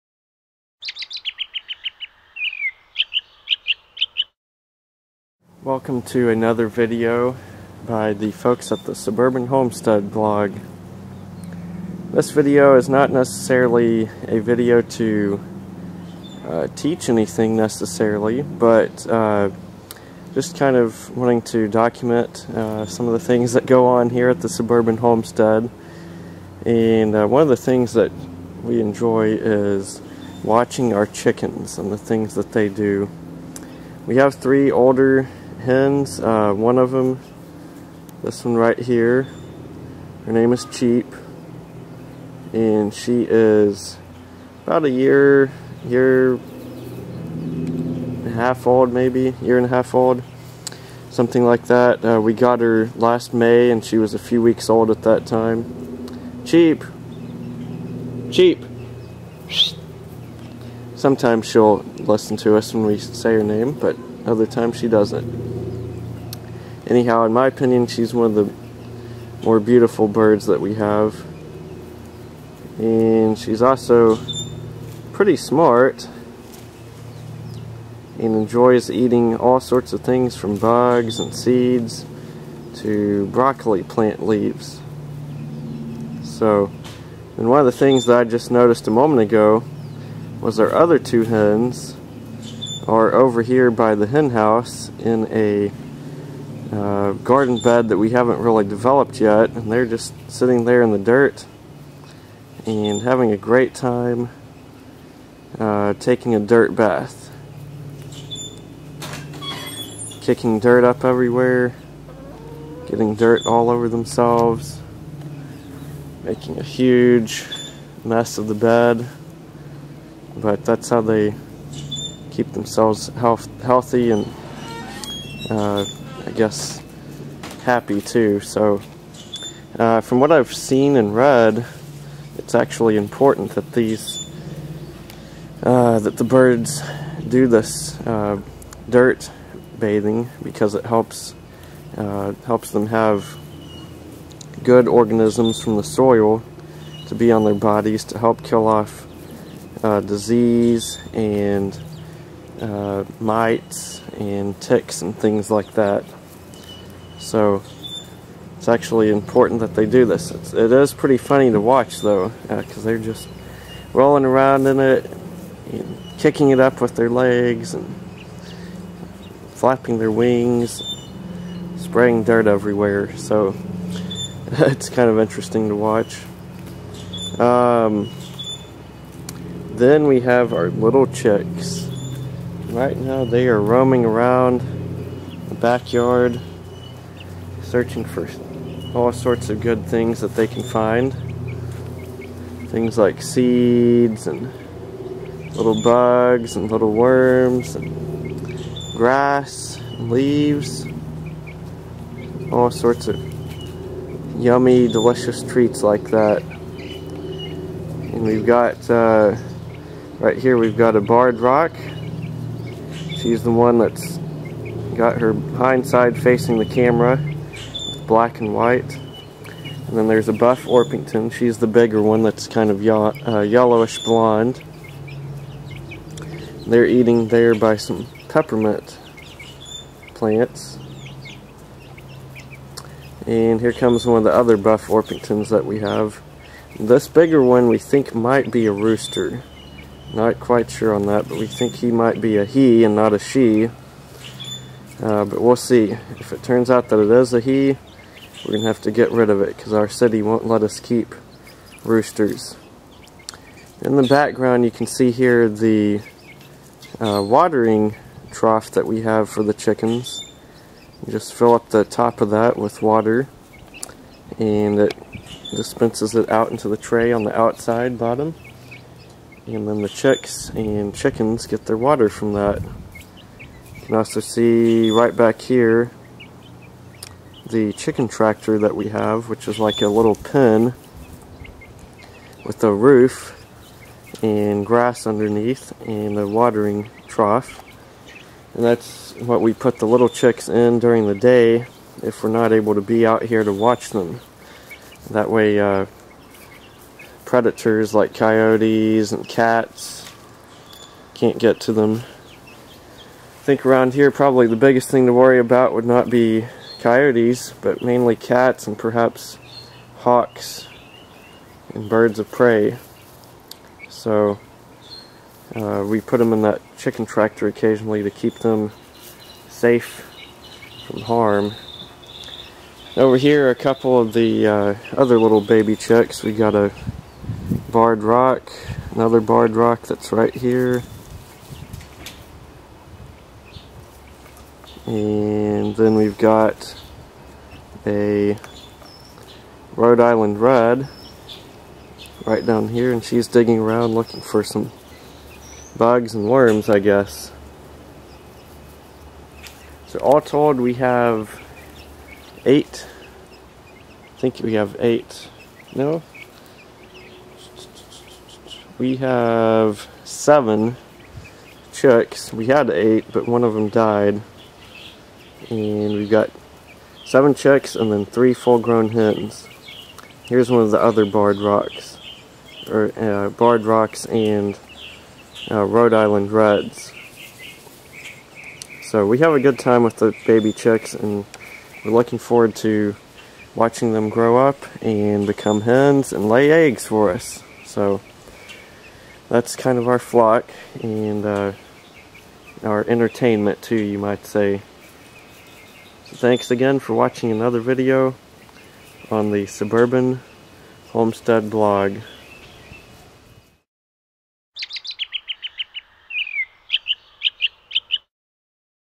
Chicken Farmers
The adventures that come with raising chickens in your backyard!